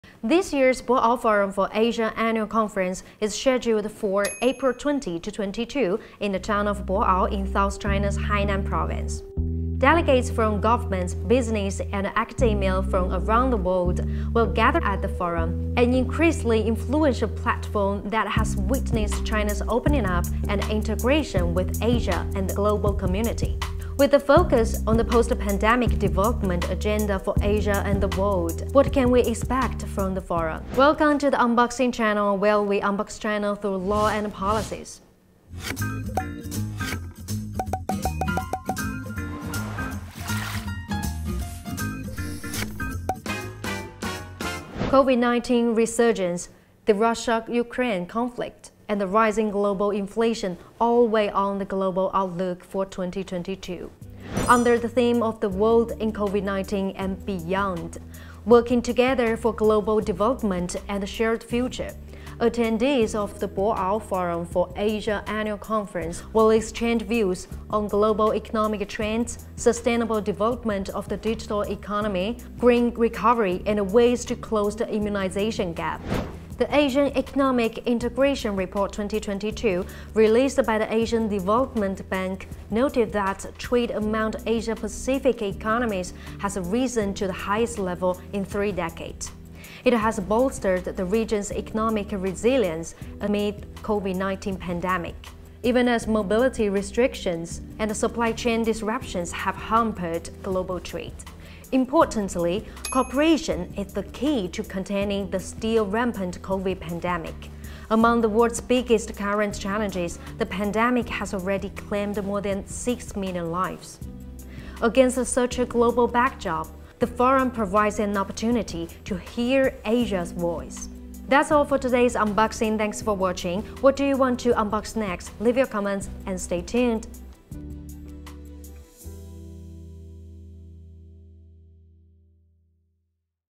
Unboxing: What to expect from 2022 Boao Forum for Asia?
This year's Boao Forum for Asia Annual Conference is scheduled for April 20-22 in the town of Boao, south China's Hainan Province. International delegates from governments, business, and academia will gather at the forum, an increasingly influential platform that has witnessed China's opening-up and integration with Asia and the global community.

With focus on the post-pandemic development agenda for Asia and the world, what can we expect from the forum?